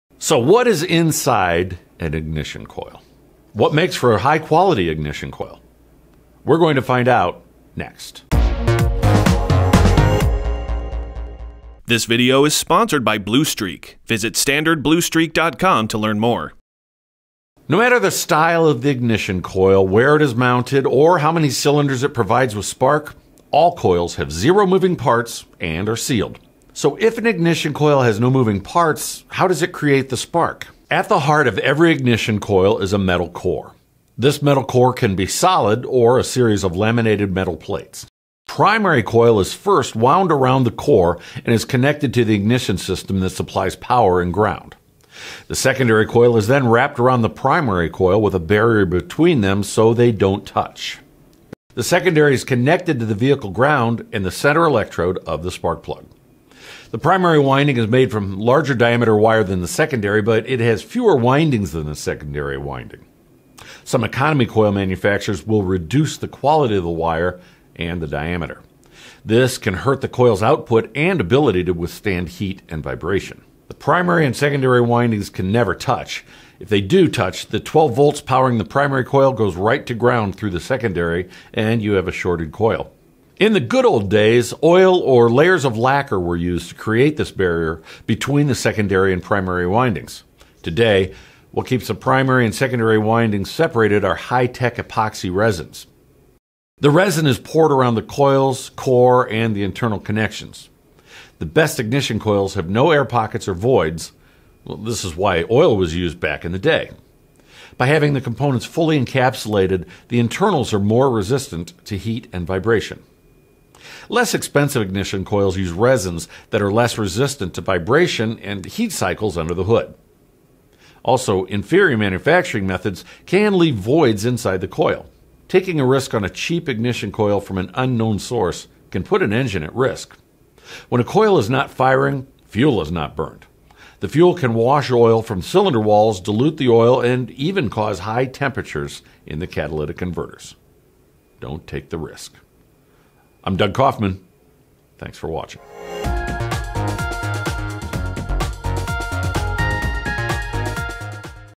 What Makes For A High-Quality Ignition Coil? | Tech Minute
No matter the style of the ignition coil, where it is mounted, or how many cylinders it provides with spark, all coils have zero moving parts and sealed. Less expensive ignition coils use resins that are less resistant to vibration, heat and endless heat cycles under the hood. Taking a risk on a cheap ignition coil from an unknown source can put an engine at risk.